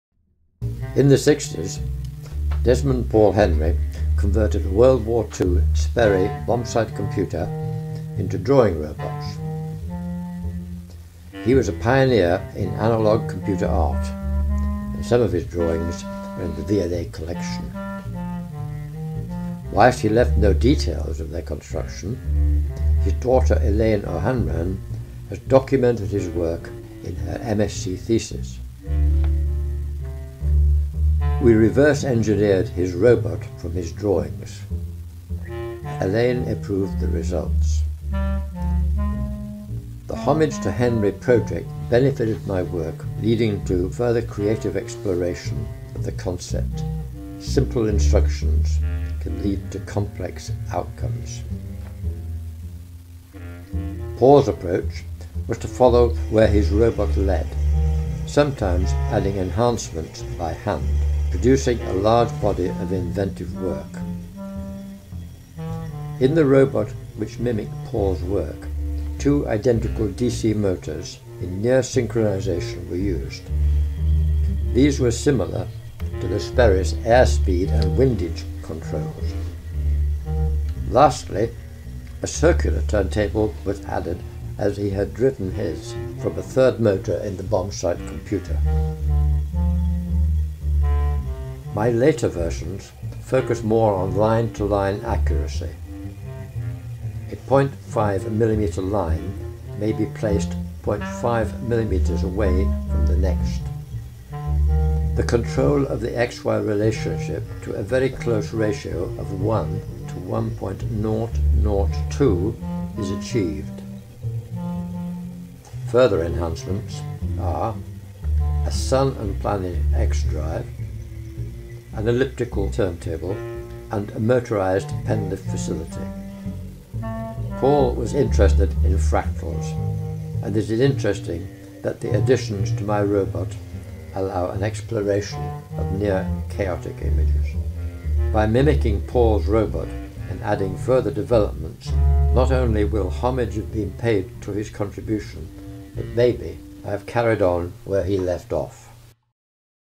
Desmond Paul Henry drawing machine 1960
Drawing robot reverse engineered to mimic the WW2 bombsight computer art works of Desmond Paul Henry. this led to extensive developments of my work on drawing robots and allowed sufficient accuracy to explore images on the edge of chaos.